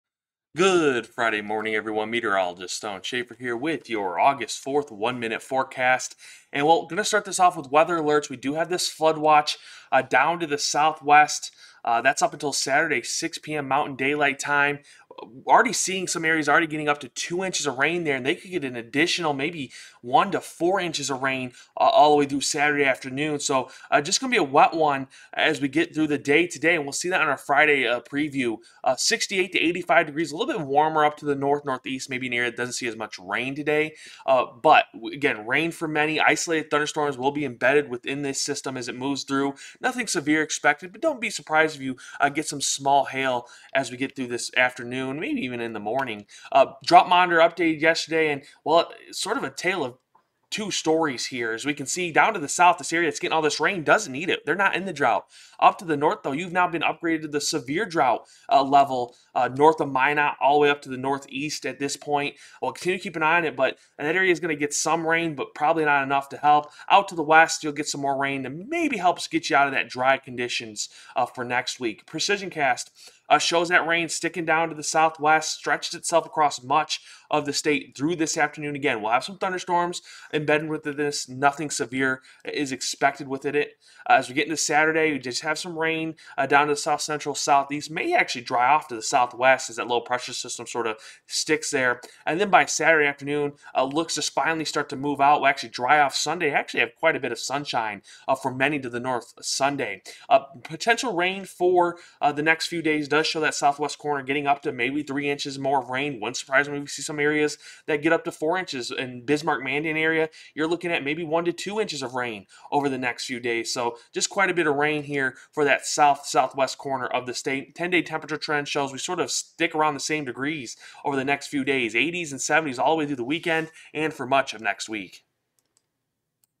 Stone's Friday Morning OneMinuteForecast 8/4
Rain started falling last night and will continue to fall until Saturday